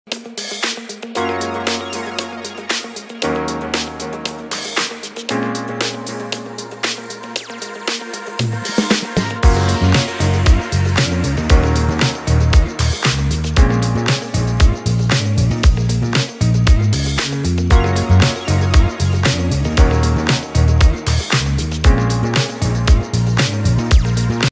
Kladivový mulčovač DelMorino Funny a najmenší traktor z radu Kioti CS2220
Táto šikovná zostava jednoduchého kladivového mulčovača DelMorino Funny a najmenšieho traktora z radu Kioti CS2220 pomáha v jablkovom sade neďaleko Novej Bane.